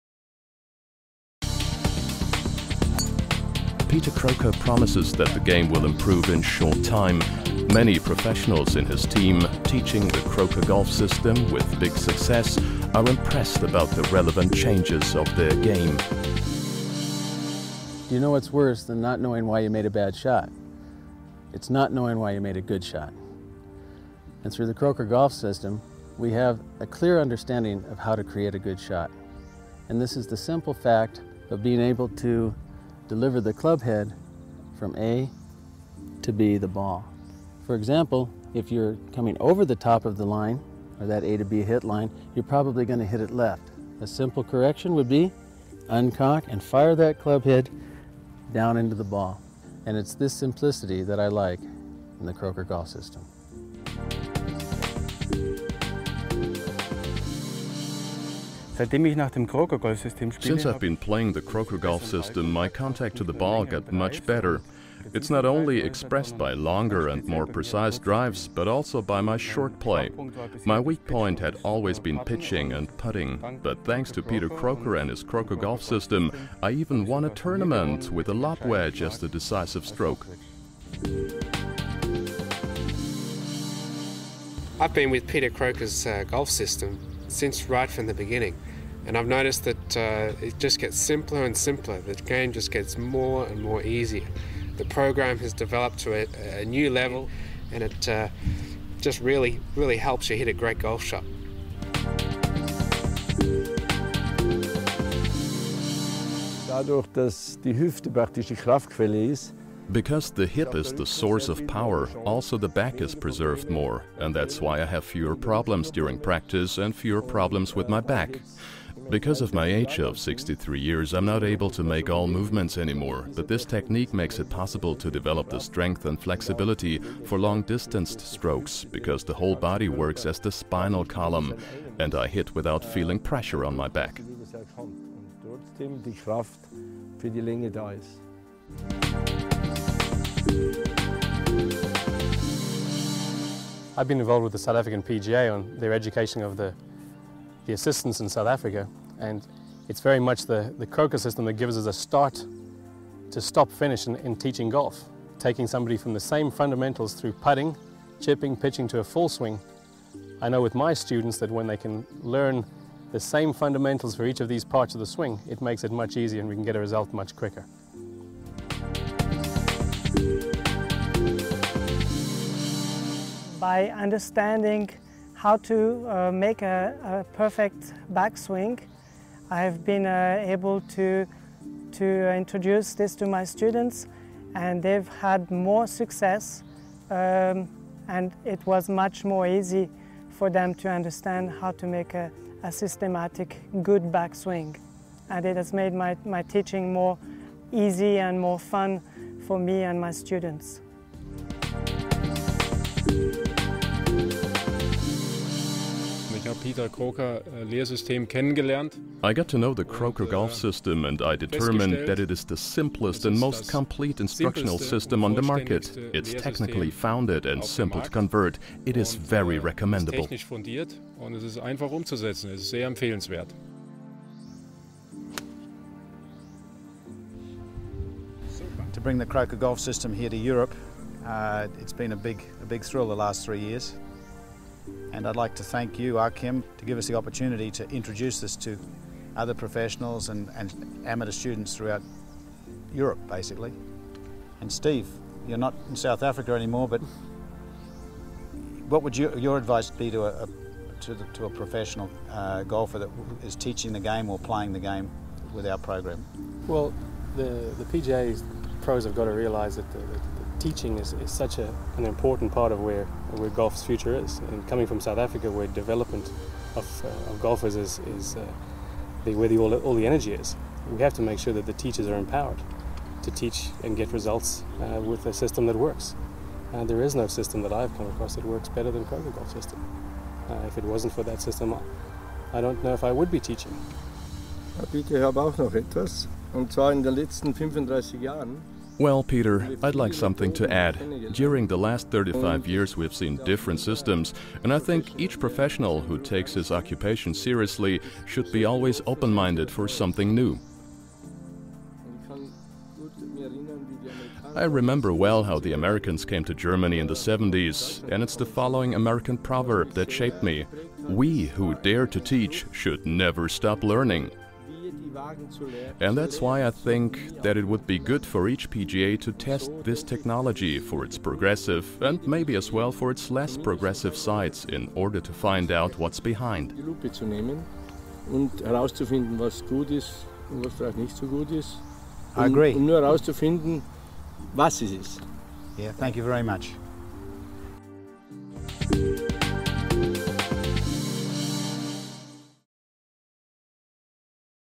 PGA golf professionals recommend Croker Golf System
To start improving your golf game no matter what your level is, visit us | PGA professionals from all over the world, USA, Australia, Europe and South Africa recommend the Croker Golf System .

Get personalized coaching and access over 300 training videos inside the Croker Golf System Coaching Community.